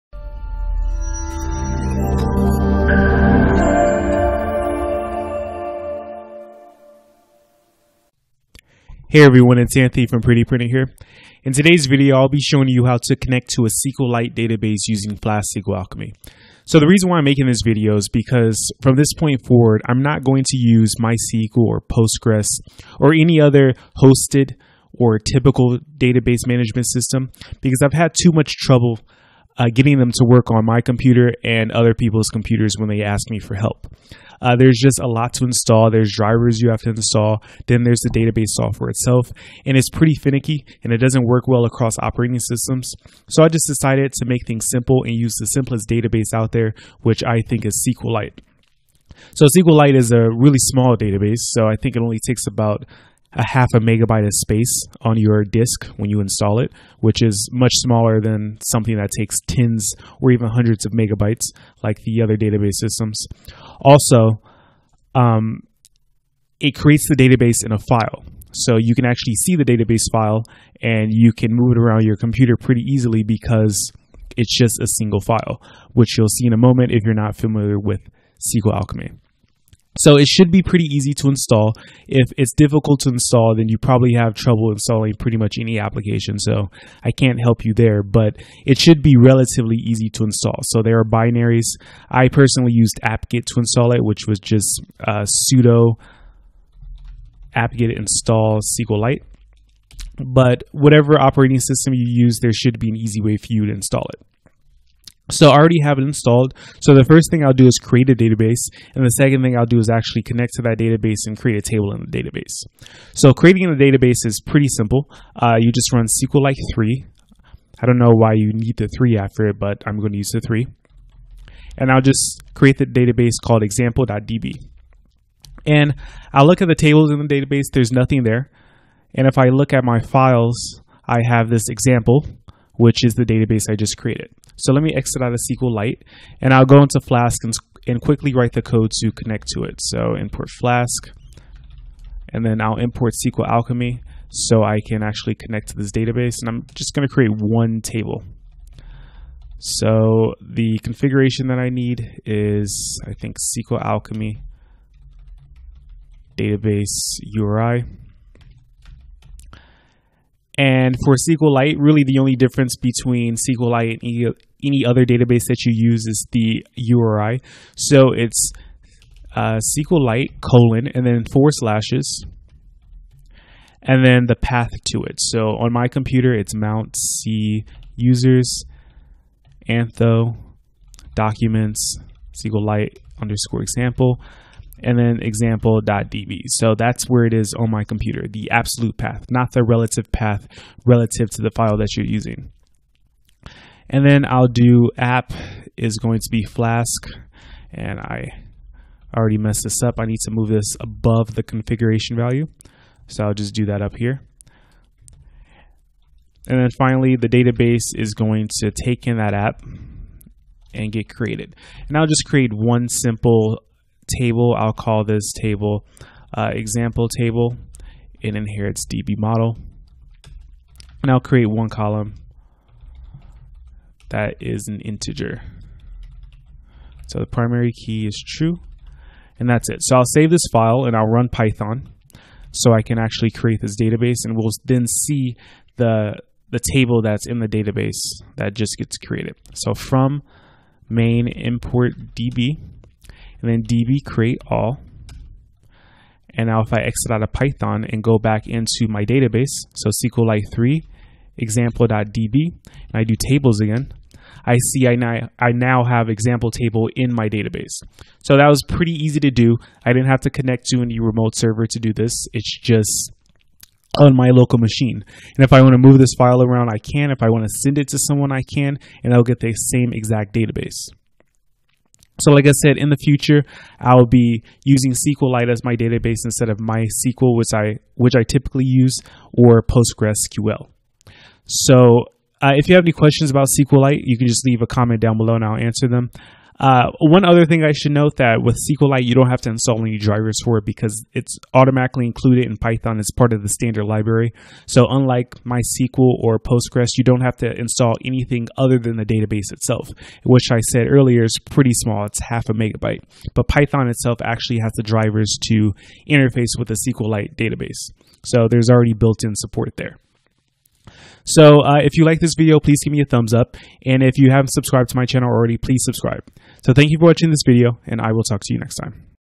Connecting to a SQLite Database Using Flask-SQLAlchemy
This video demonstrates how to create a SQLite database and connect to it using Flask-SQLAlchemy.